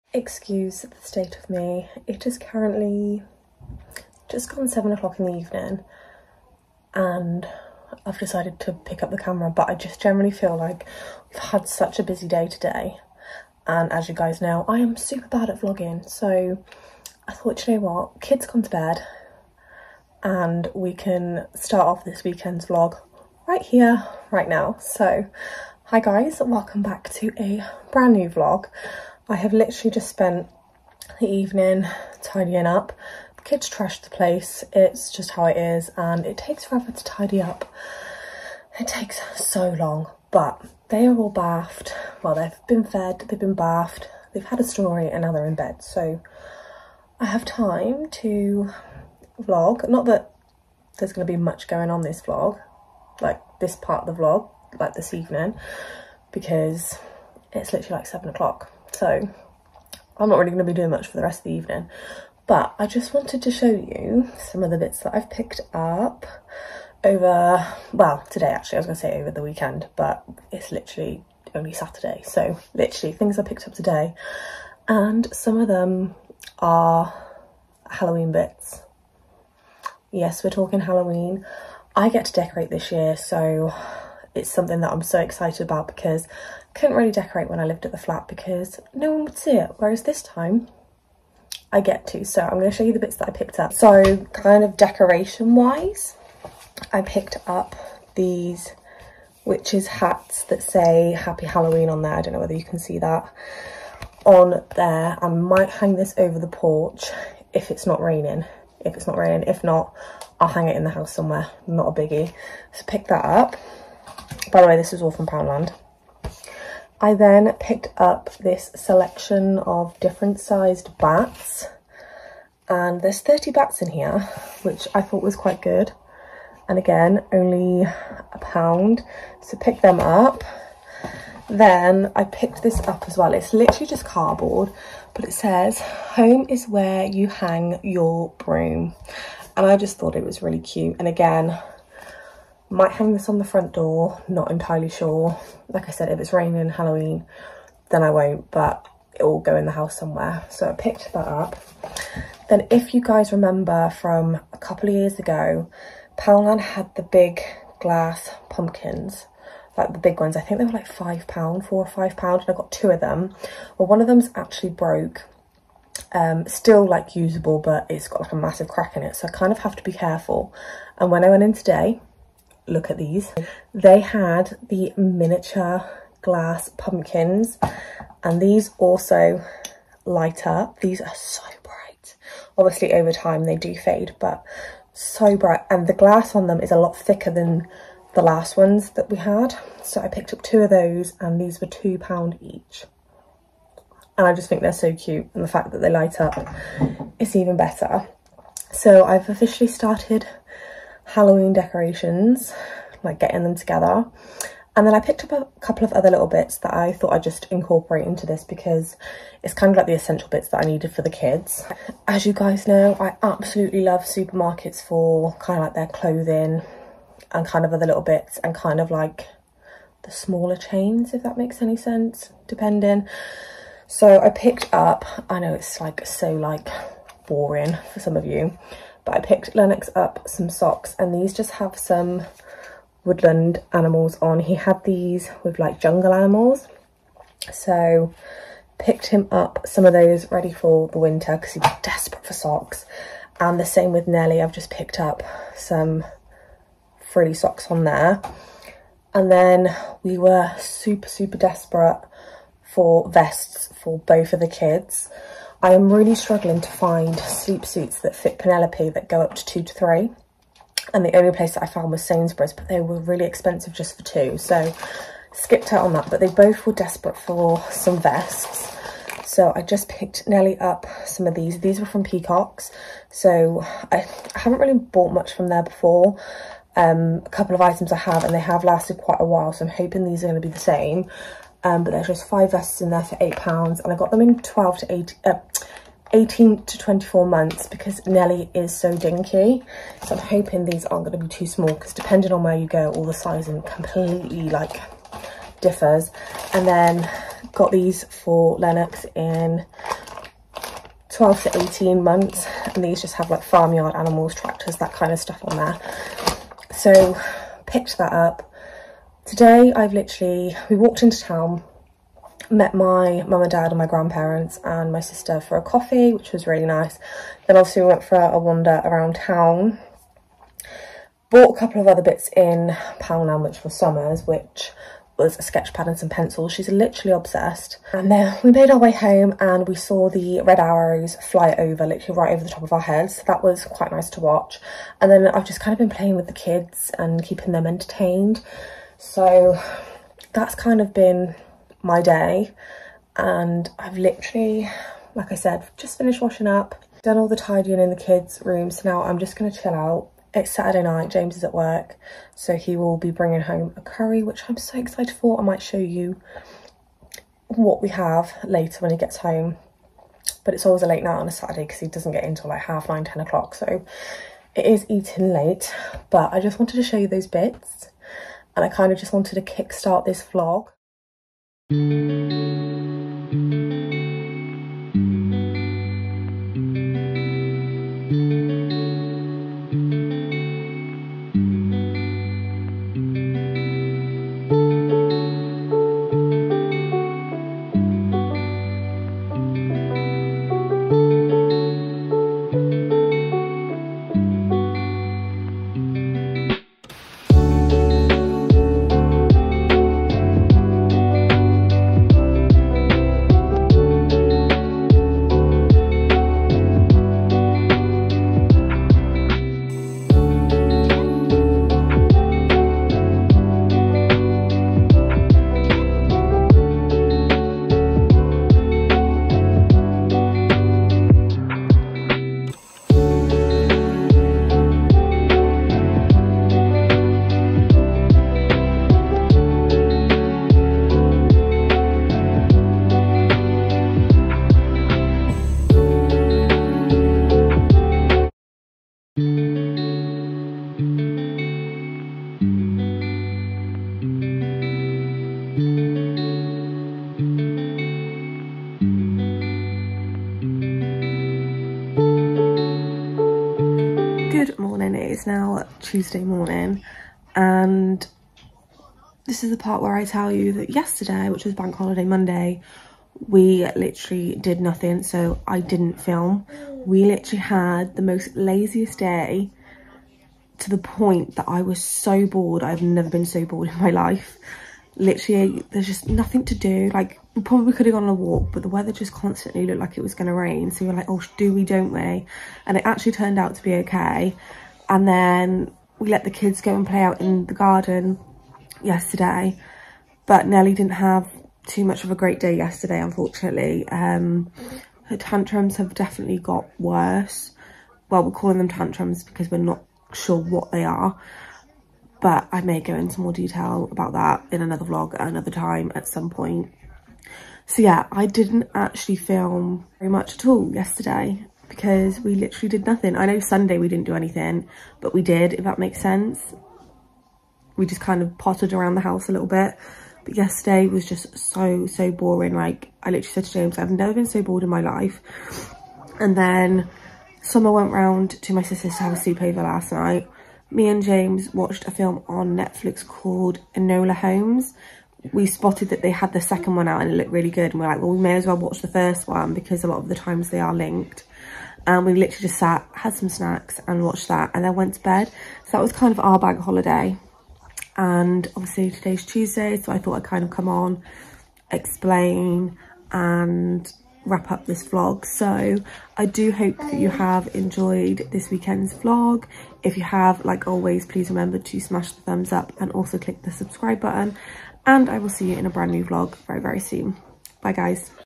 BANK HOLIDAY WEEKEND | Mini Haul & Cosy Days | Amy Elizabeth
Bank holiday weekends are normal super busy but for once we had a very chilled one.
There is also a mini haul including Poundland, Morrisons and peacocks.

Love to you all x
Amy Elizabeth